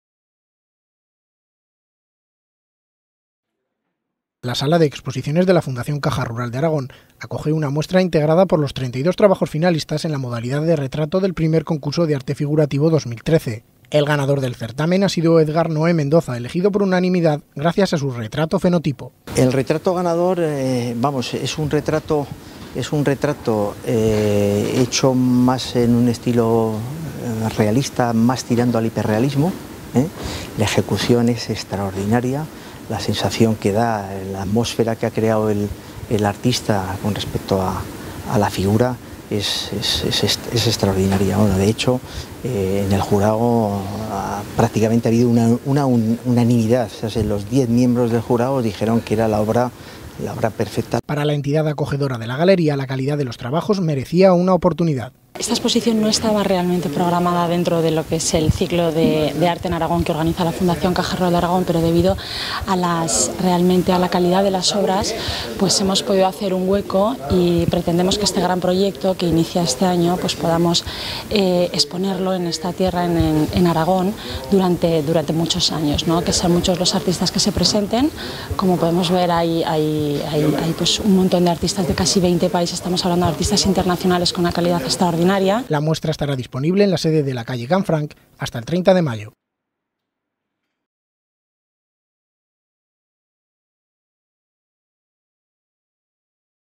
La Sala de Exposiciones de la Fundación Caja Rural se llena de miradas y rostros
(07/05/2014) Los 32 trabajos finalistas del I Concurso de Arte Figurativo - ModPortrait 2013 ocuparán la Sala de Exposiciones de la Fundación Caja Rural de Aragón hasta el 30 de mayo. En esta muestra colaboran la Fundación de las Artes y los Artistas y el Museo Europeo de Arte Moderno de Barcelona. El mexicano Edgar Noé Mendoza ha sido el ganador.